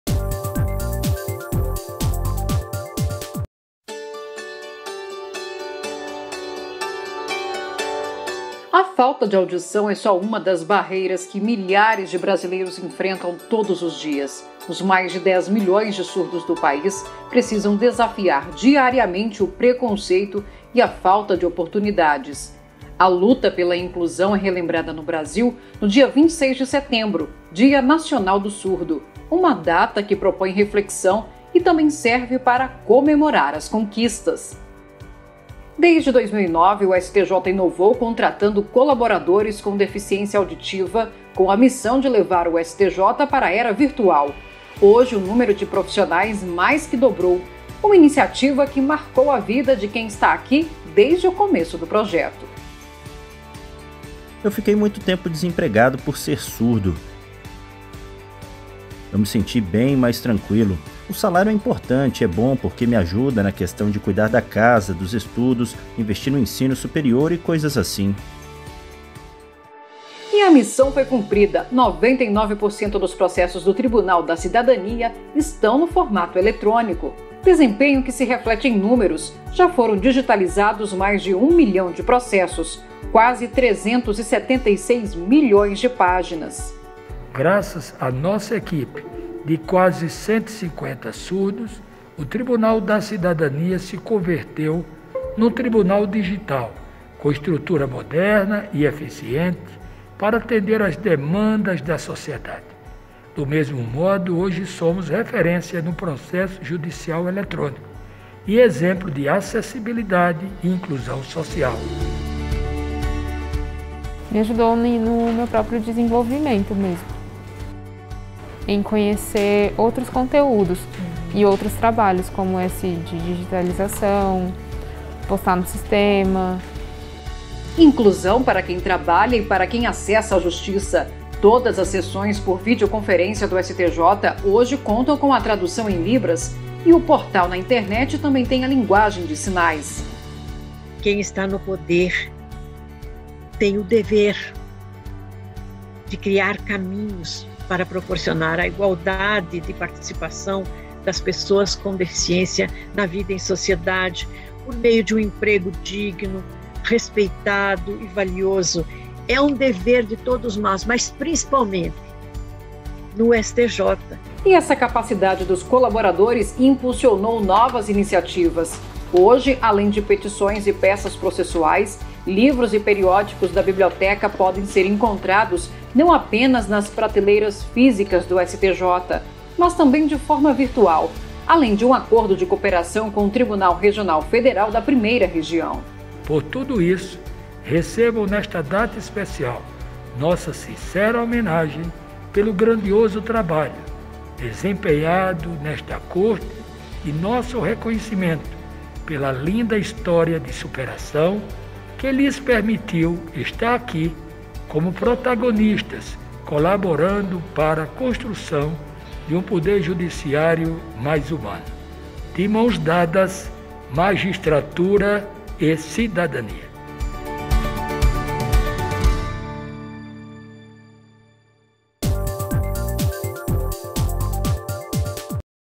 STJ homenageia colaboradores no Dia Nacional dos Surdos, comemorado neste sábado (26)
Amanhã, 26 de setembro, é o Dia Nacional do Surdo. Data de criação da primeira escola de surdos do Brasil, em 1857. No Superior Tribunal de Justiça,  os  profissionais que possuem deficiência auditiva e contribuem com o funcionamento da corte receberam uma homenagem especial.